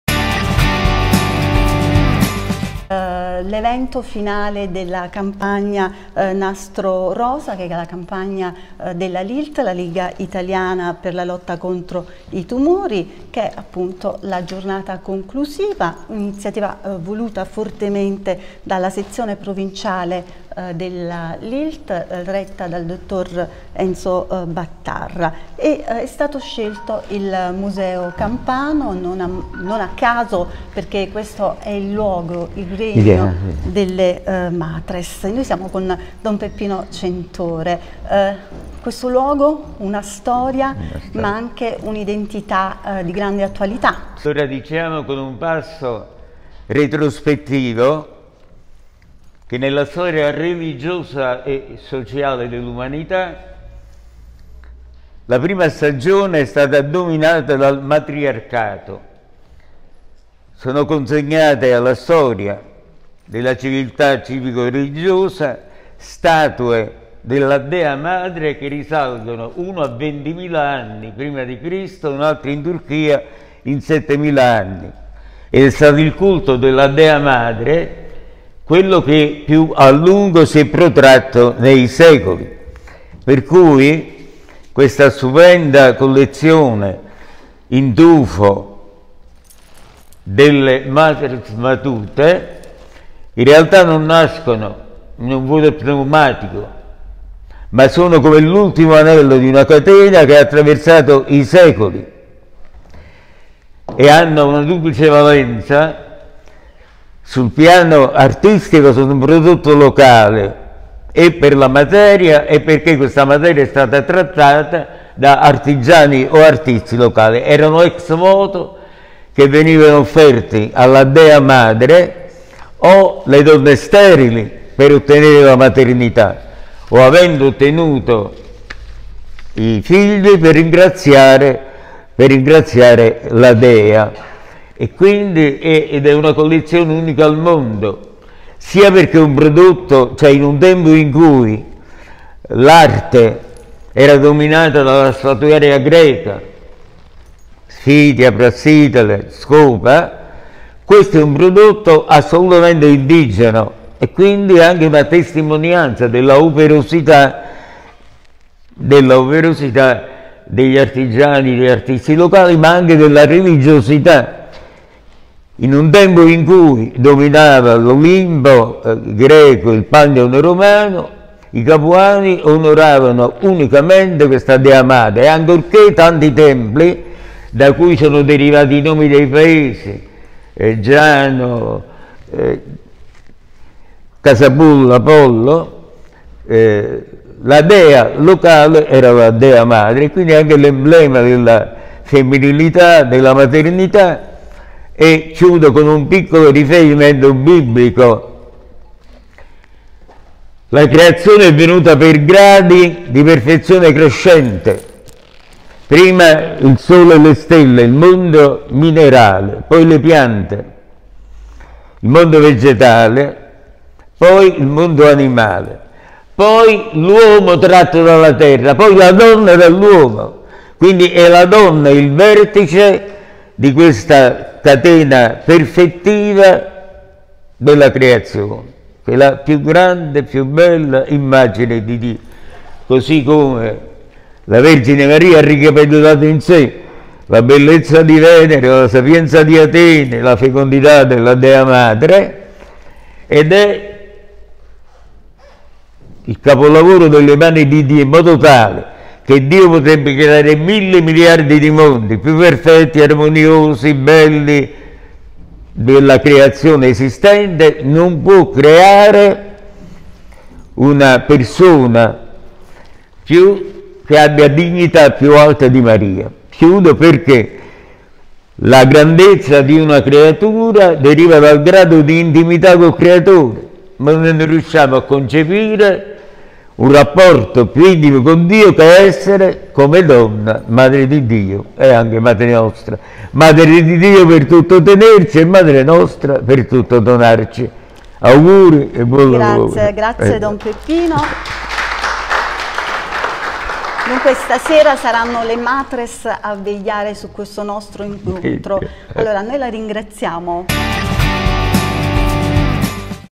Museo Campano. Le Matres Matutae, Don Giuseppe Centore
Una sera con le Matres per la Lilt, lega tumori al termine della campagna di prevenzione. Intervista a don Giuseppe Centore.